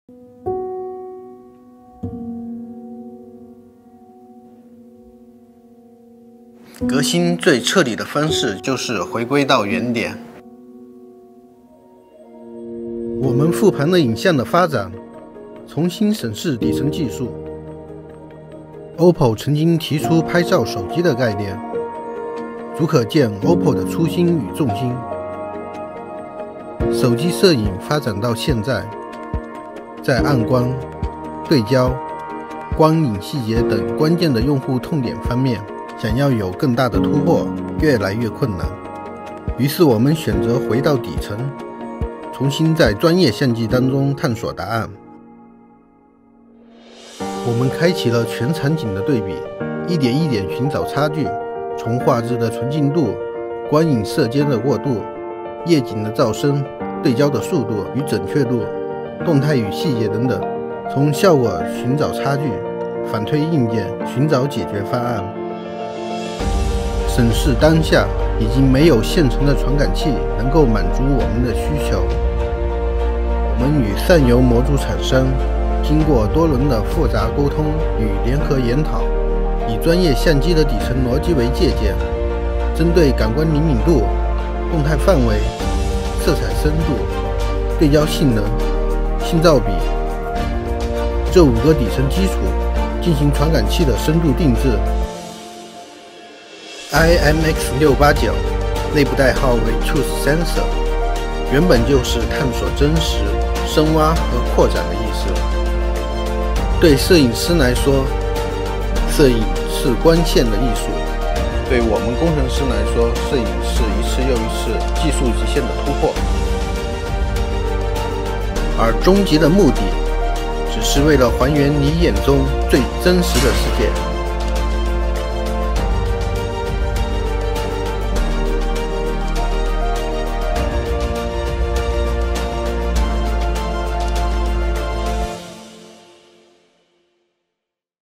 OPPO Find X2 Series- Our Camera History -Don Tang | OPPO Global
This is video to talking about the camera inside the OPPO Find X2 series smartphones with Don Tang speaking in Chinese but transcript is in English.